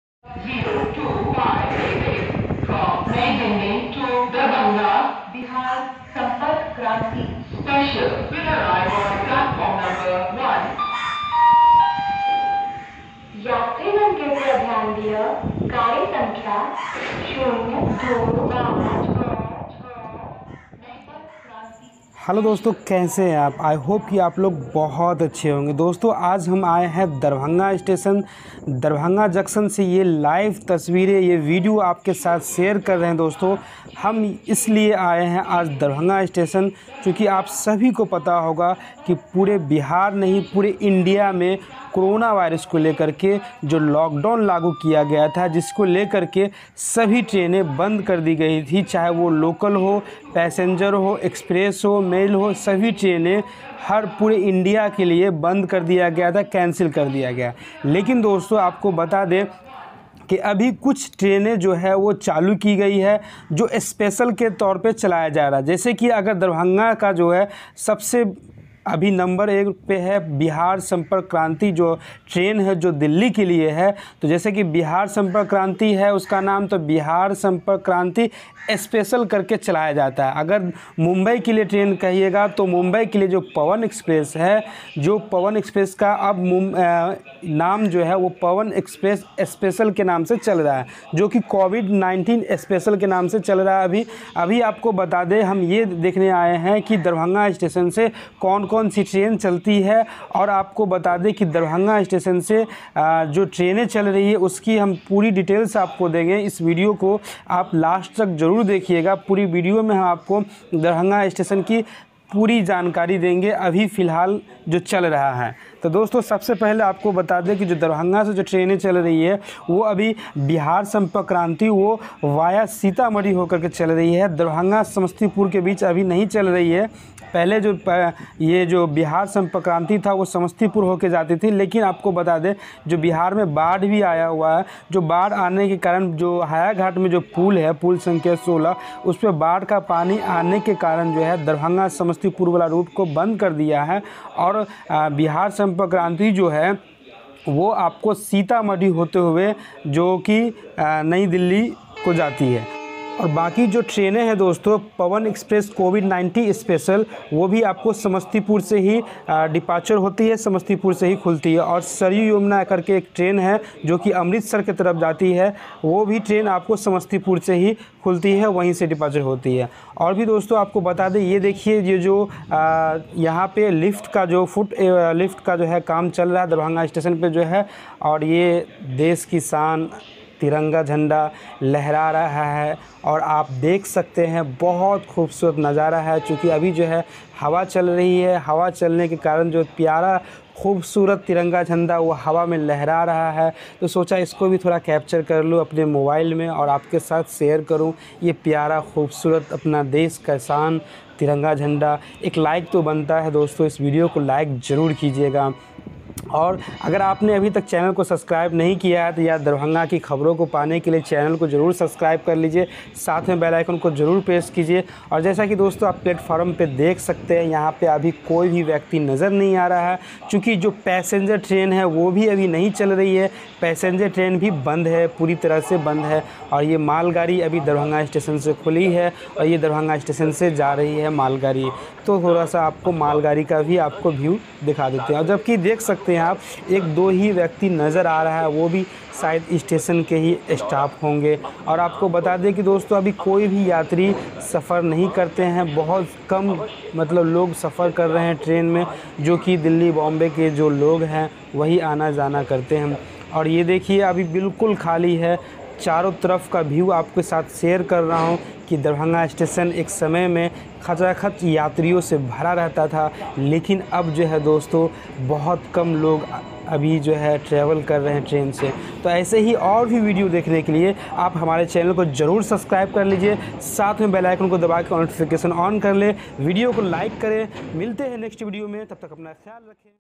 दरभंगा स्टेशन का यह अद्भुत नजारा लॉक डॉन से पहले आपने कभी नहीं देखा होगा।Darbhanga|Bihar|Railway Sta
दरभंगा स्टेशन का यह अद्भुत नजारा लॉक डॉन से पहले आपने कभी नहीं देखा होगा।

This amazing view of Darbhanga station you have never seen before Lock Dawn.